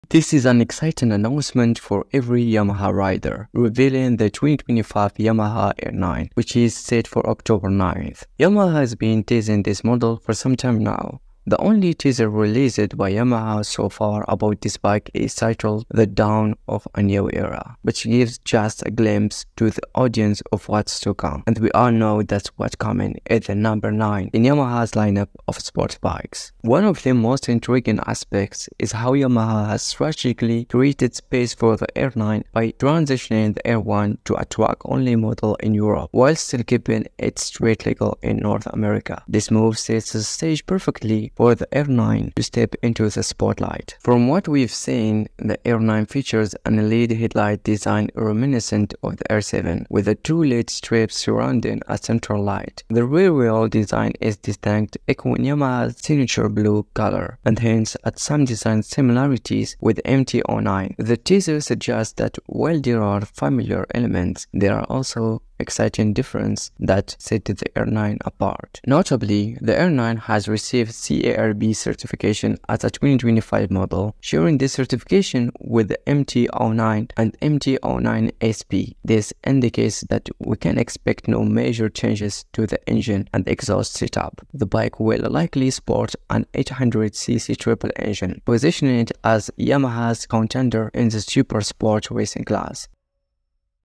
Countdown to the Yamaha YZF-R9 Reveal: What to Expect on October 9
In this video, we discussed various insights and assumptions regarding the features we can expect from the Yamaha R9, based on its teaser announcement.

Yamaha.